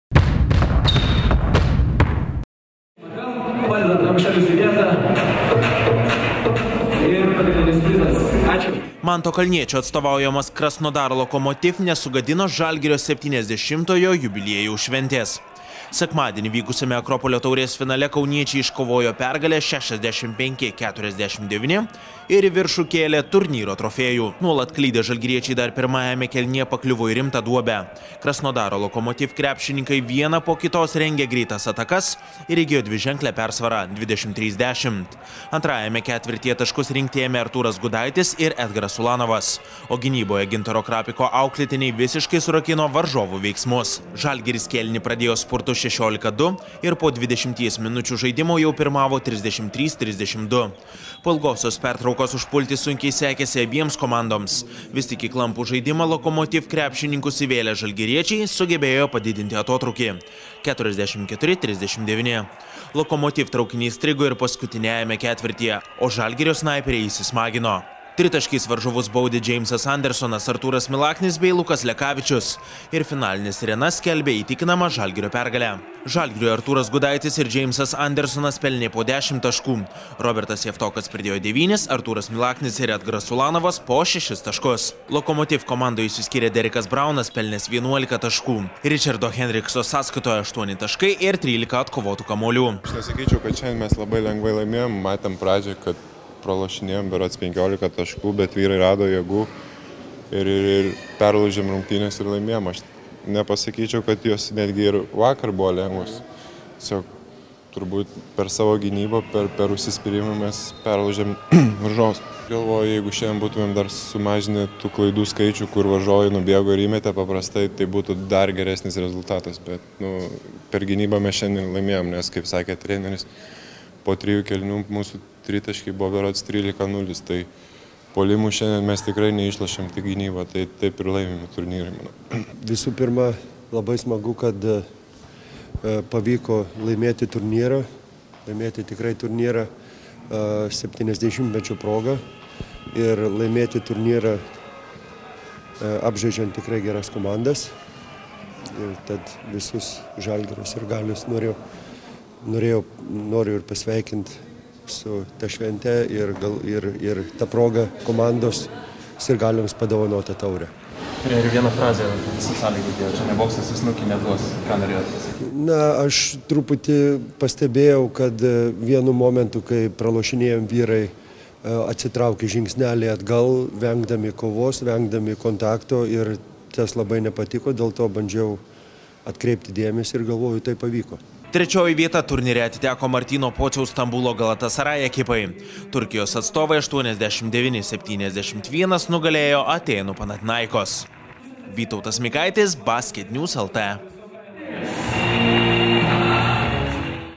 Geležinę gynybą parodęs „Žalgiris“ iškovojo „Akropolio“ taurę
Po itin nesėkmingo pirmojo kėlinio atsitiesę Kauno „Žalgirio“ krepšininkai surakino Krasnodaro „Lokomotiv“ puolimą ir džiaugiasi iškovota „Akropolio“ taure. Svečiams iš Rusijos nepavyko sugadinti „Žalgirio“ 70-ojo jubiliejaus šventės ir kauniečiai iškovojo pergalę 65:49 (10:23, 23:9, 11:7, 21:10).